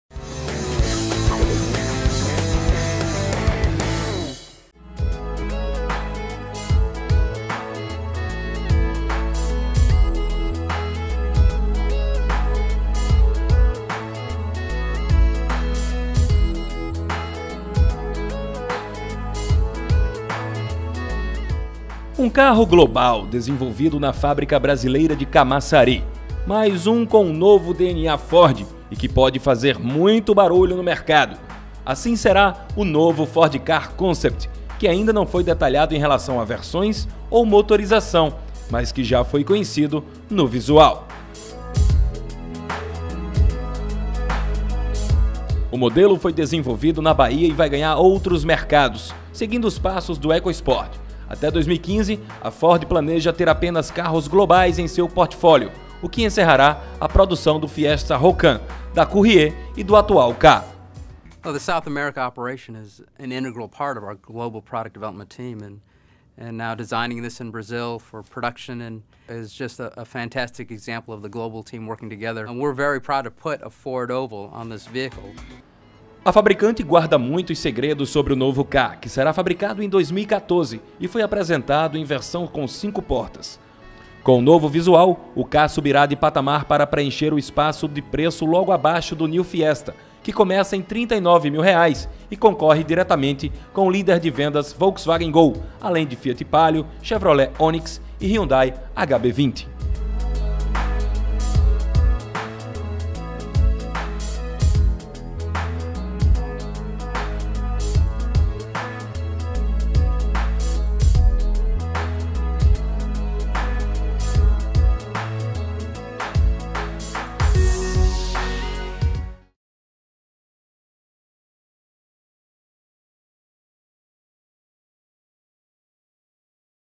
Novo KA Concept Ford 2014 - Motores & AÇÃO - Carro Global
A Ford apresentou o Ka Concept, protótipo que dará origem à nova geração do compacto Ka. Novo compacto da marca seda lançado ate março no Brasil. O que achou?
Carro será fabricado em Camaçari, na Bahia.